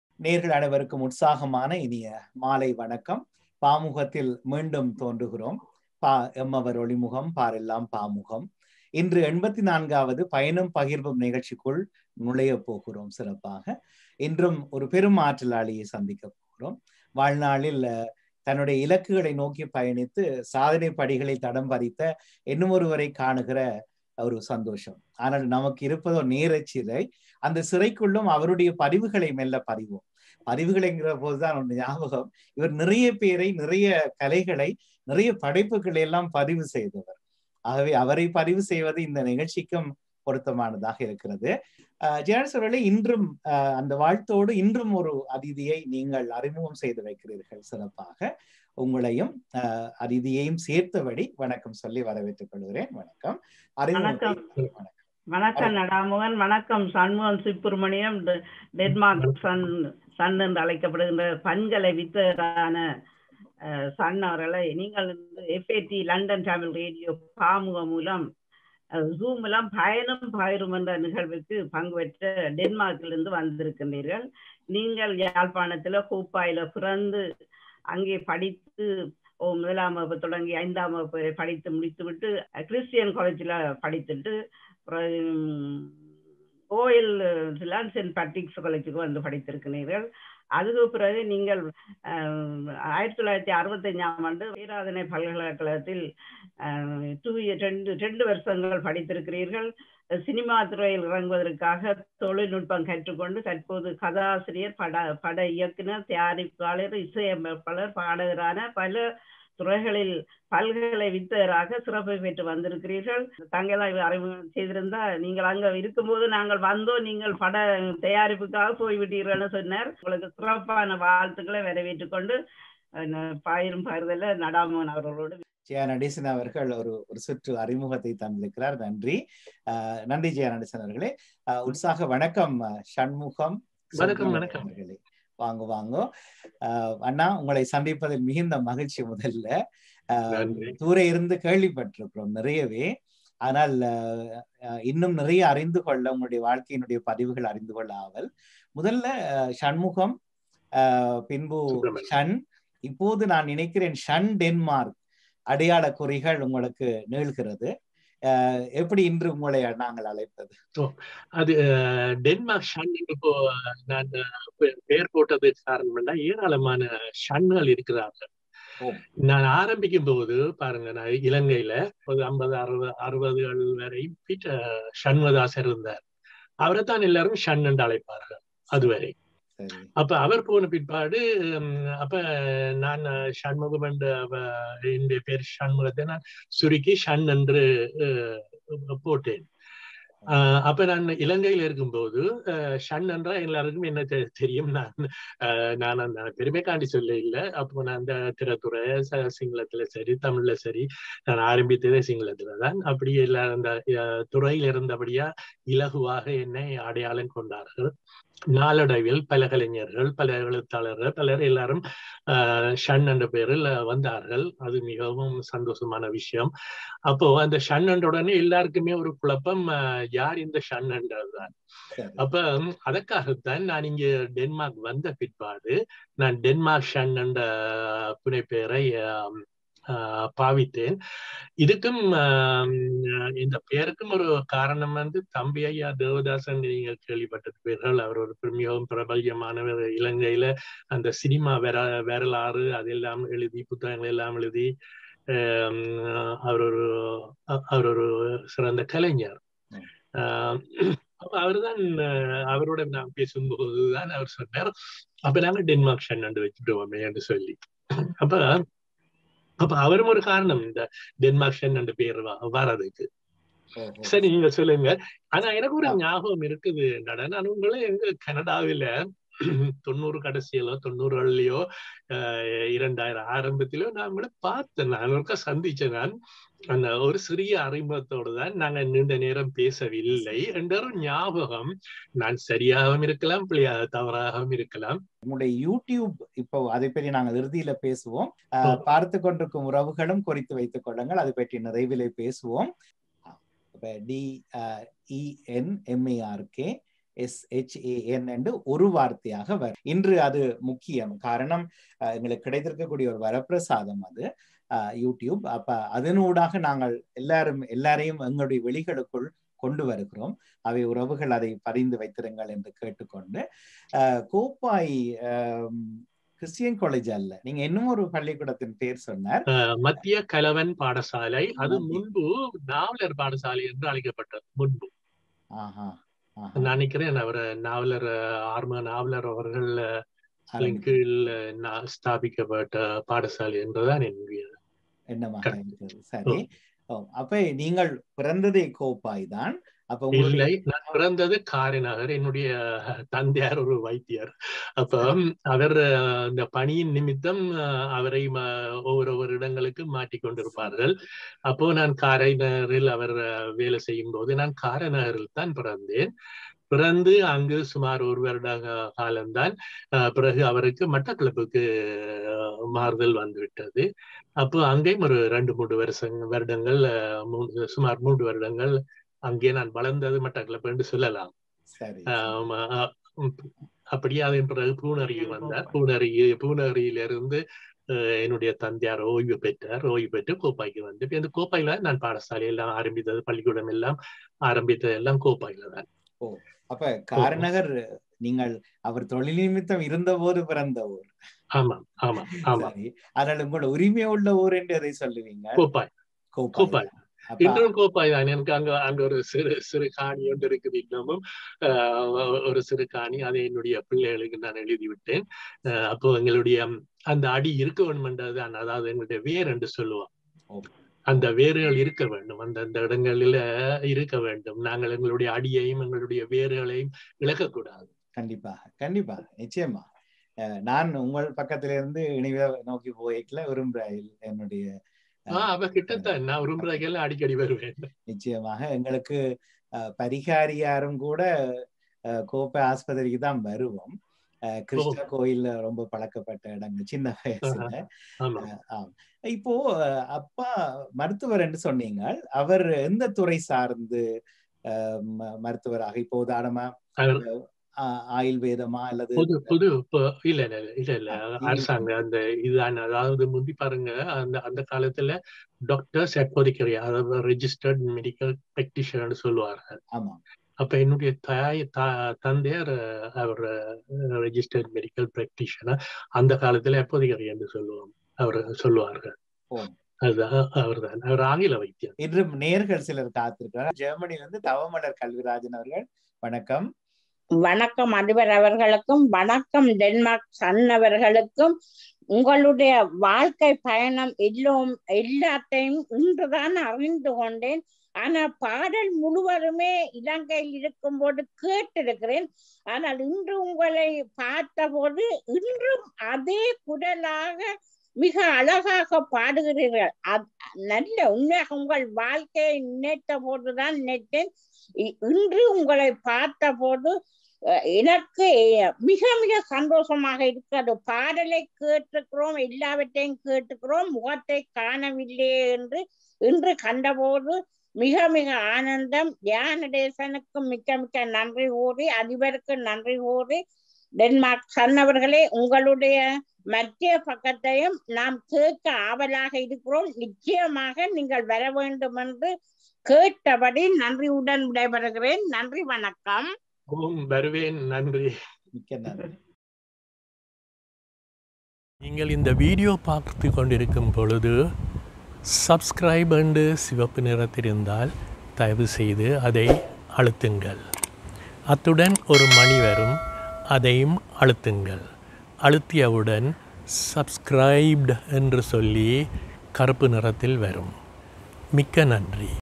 Part1 Denmarkshan#Nada Mohan#Interview#London Tamil Radio#டென்மார்க்சண்லண்டன் தமிழ் வானொலி#
Part 1 - Denmarkshan's Interview for London Tamil Radio. லண்டன் தமிழ் வானொலிக்கு டென்மார்க்சண் வழங்கிய நேர்காணல்.